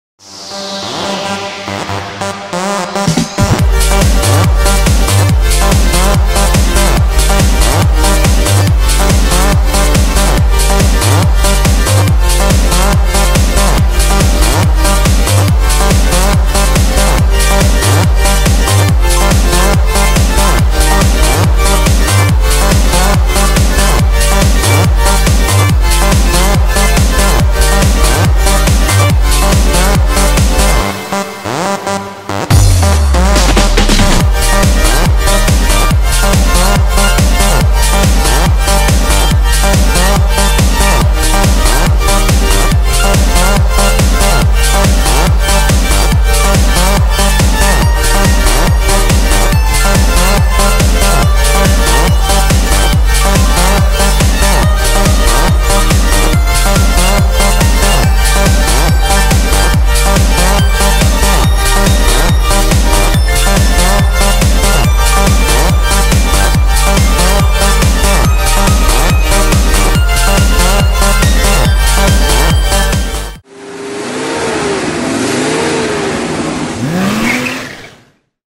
Daimler Trucks HAD Mode - Arocs HAD | AutoMotoTV
Hydraulic Auxiliary Drive is suitable for anyone who needs a short time maximum traction, but not want to give up a high payload and optimum driveline. These include Kippsattelzüge, timber transport, Construction vehicles and hook lift. HAD is available for OM 470 and OM 471 in the Actros, and Arocs Antos.